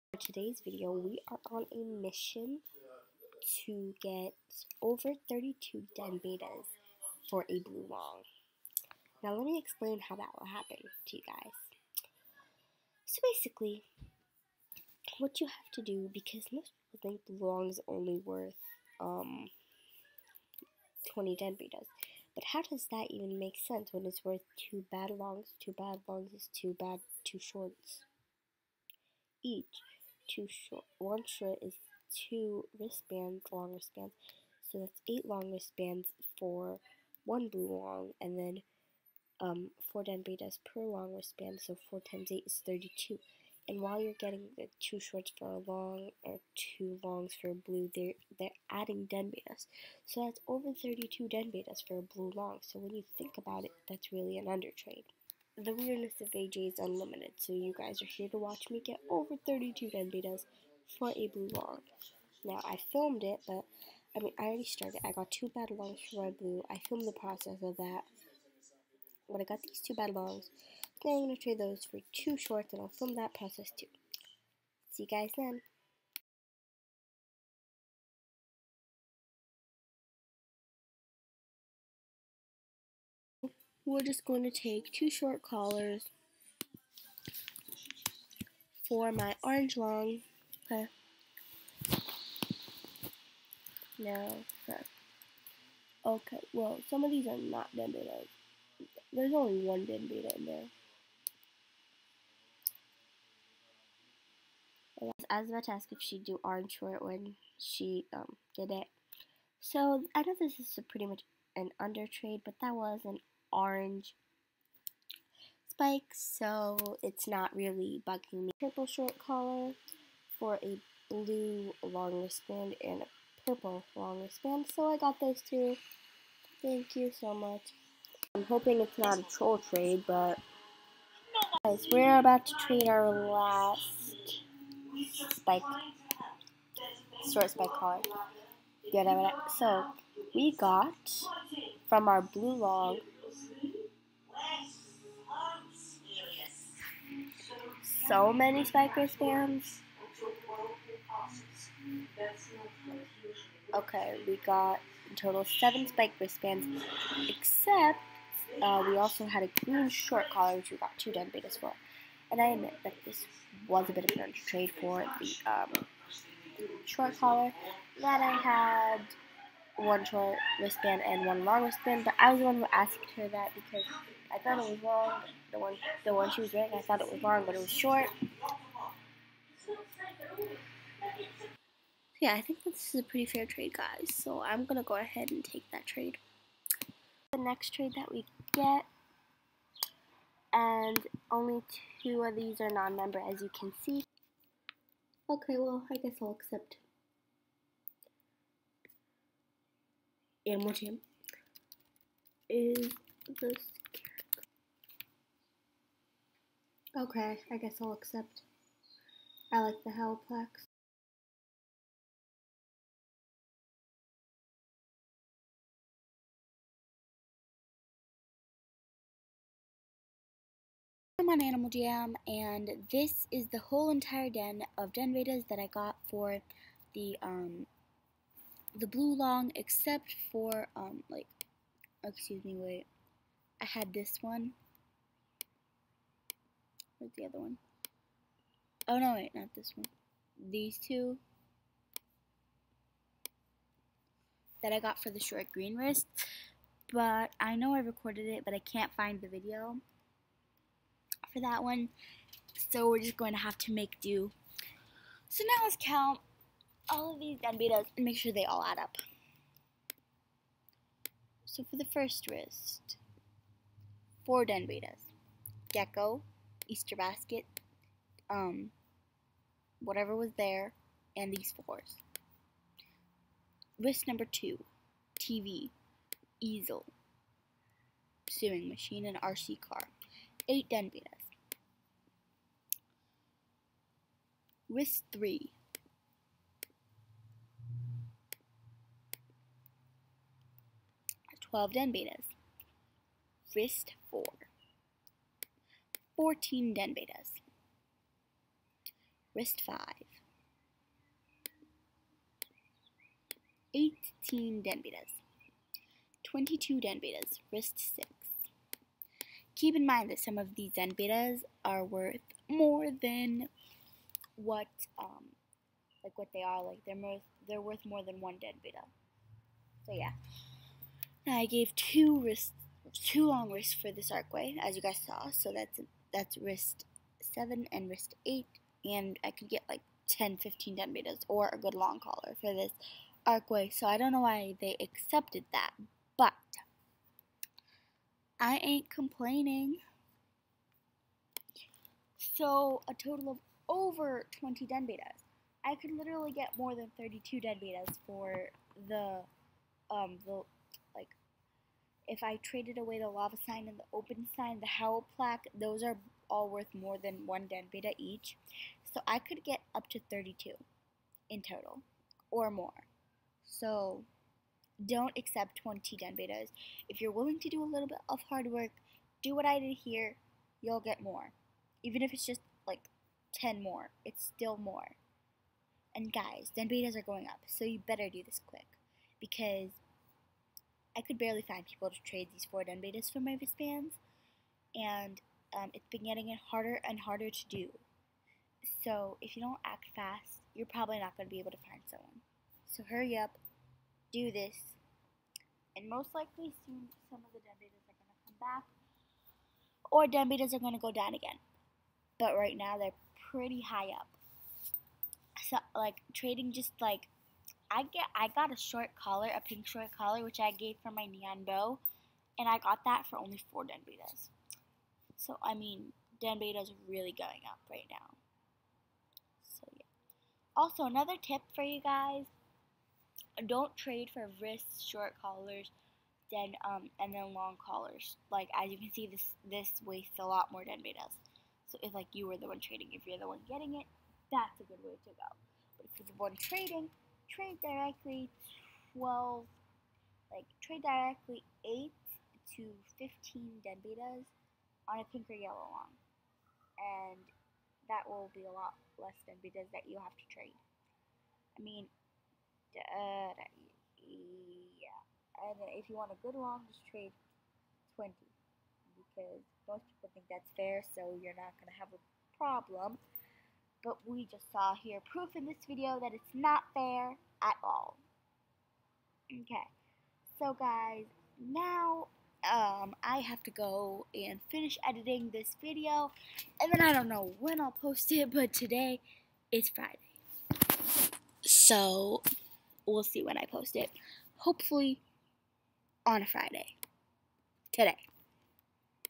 Getting Over 20 Den Betas For A Blue Long Spiked Collar.
Guys, I know that the scarecrow is a den beta, but I do not count it as one, as I don't like it that much. Also, it is one of the less rare and popular ones. I hope you guys enjoyed this video! Also, comment down video ideas and suggestions for the future, please!